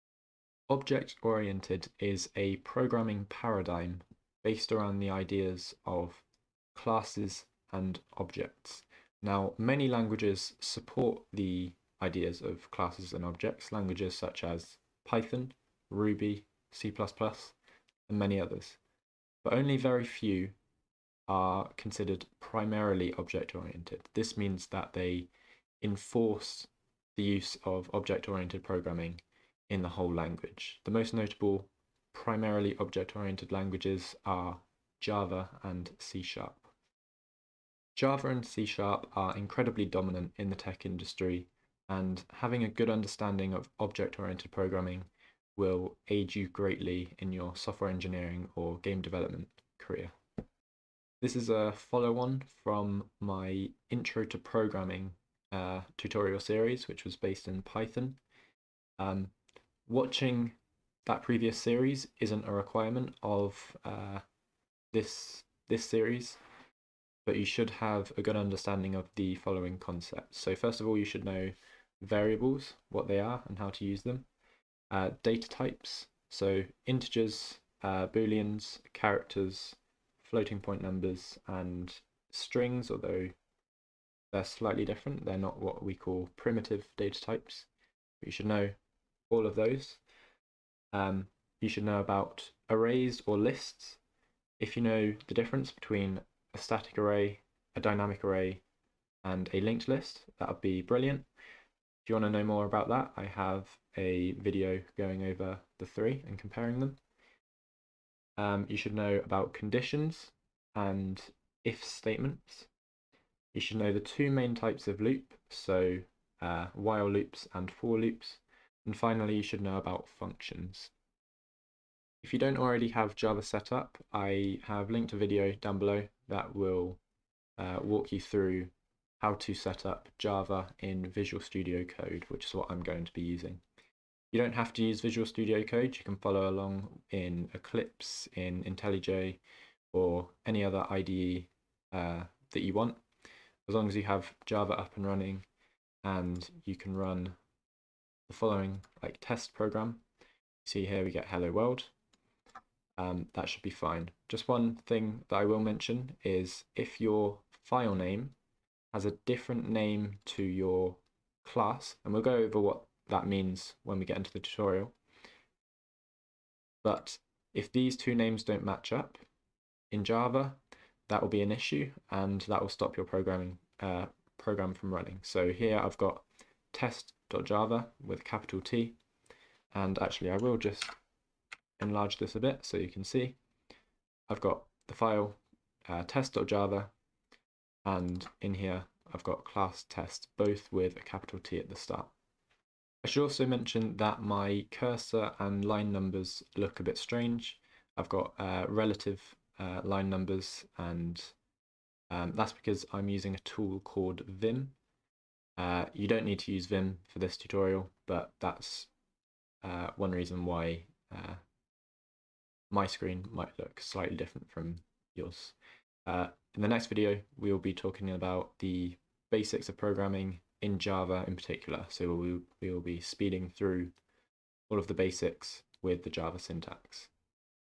Intro to OOP | #1 Prerequisite Knowledge
This series is a follow-on from Intro to Programming, and aims to introduce the most important Object-Oriented Programming concepts.